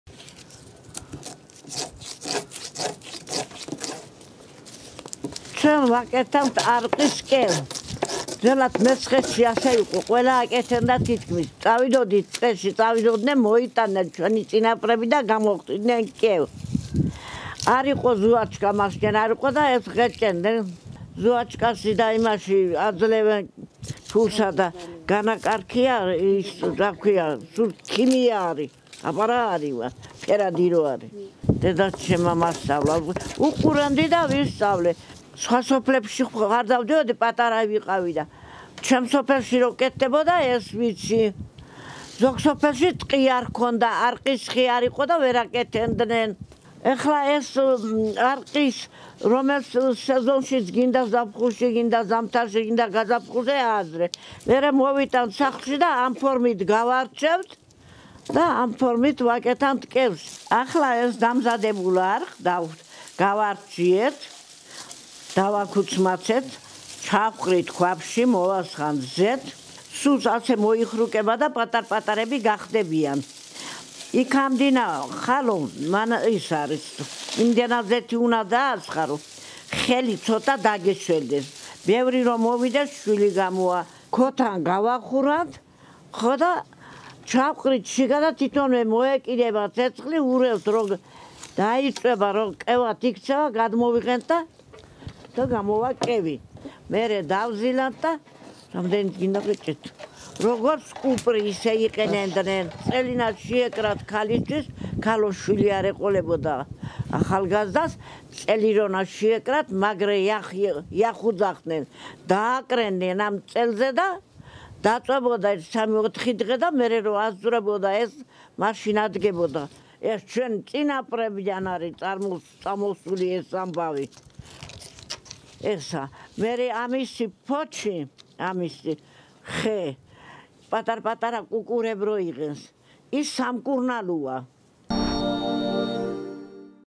მივიწყებული ტრადიცია – როგორ ამზადებს 87 წლის ევა ბაბო არყის ხიდან კევს
ასპინძაში, სოფელ ნაქალაქევში, არყის ხის ქერქისგან კევის გამოხდის ტრადიციას 87 წლის ევა მაისურაძე აგრძელებს. როგორც თავად ამბობს, ძველად, ეკოლოგიურად სუფთა საღეჭი რეზინი სამკურნალოდაც გამოიყენებოდა. უძველესი რეცეპტით დამზადებული კევი ახლა ფერად–ფერადმა „ზუაჩკებმა“ ჩაანაცვლა და მამა-პაპის დროინდელი ტრადიციაც დავიწყების გზას ადგას.